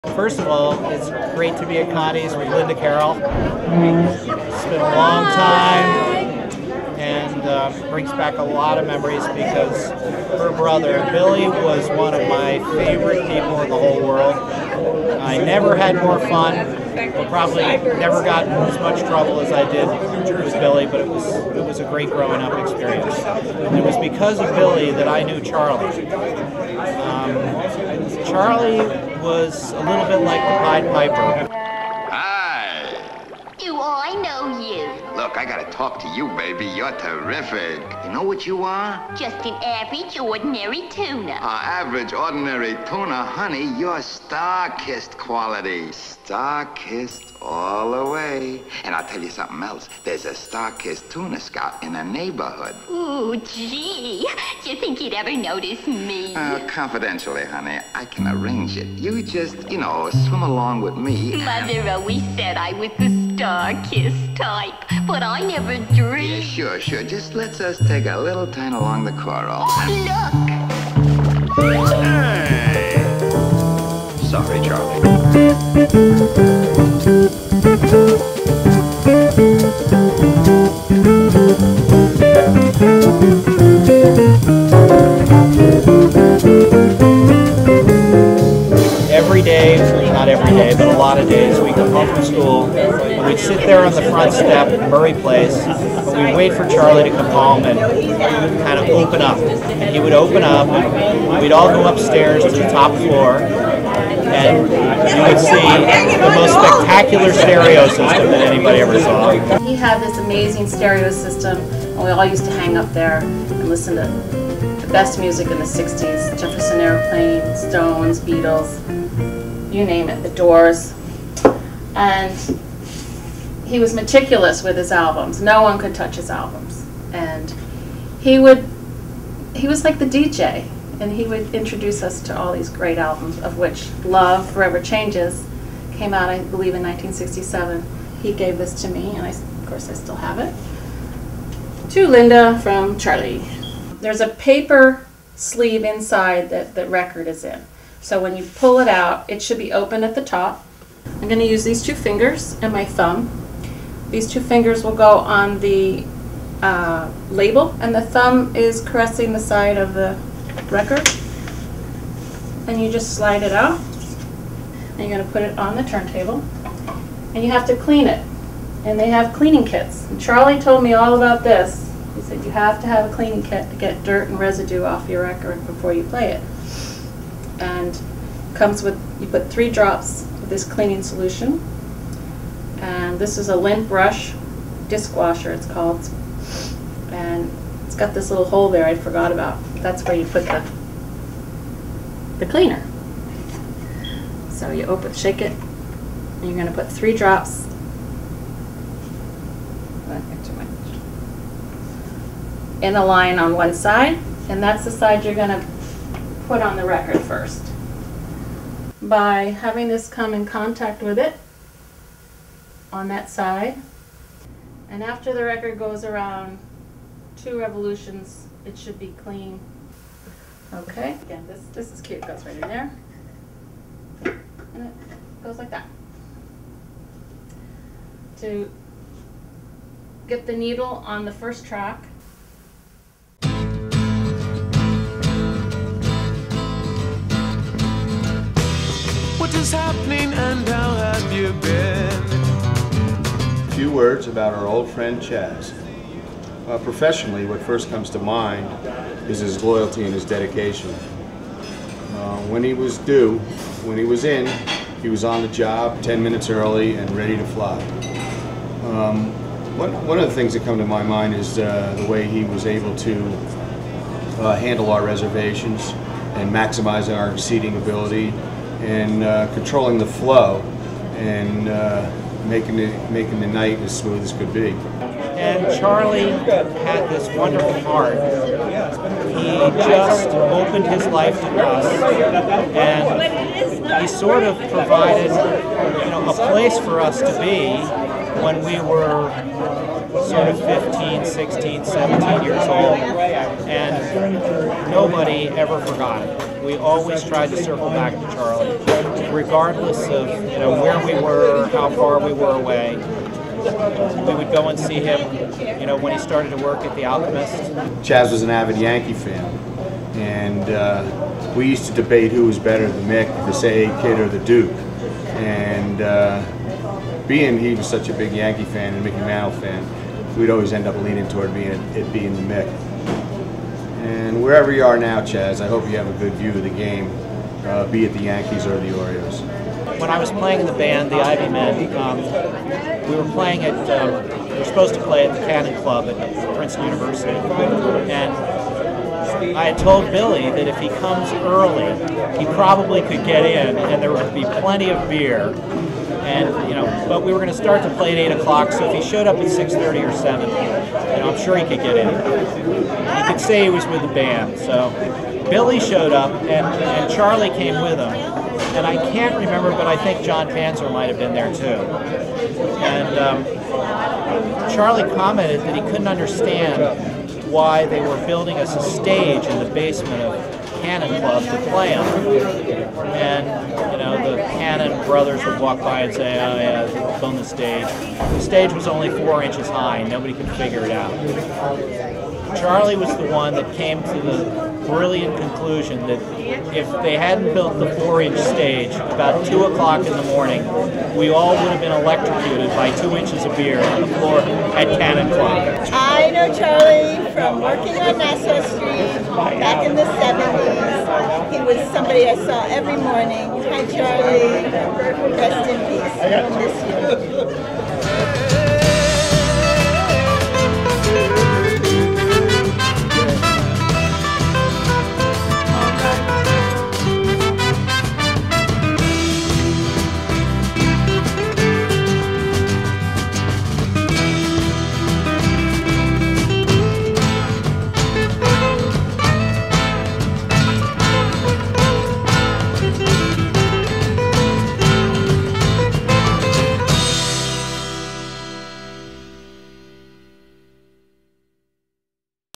Charlie Sculerati Tribute Film
Tribute video for Charles Sculerati, shown at his memorial service on June 15, 2014, at the Princeton Arts Council in Princeton, NJ.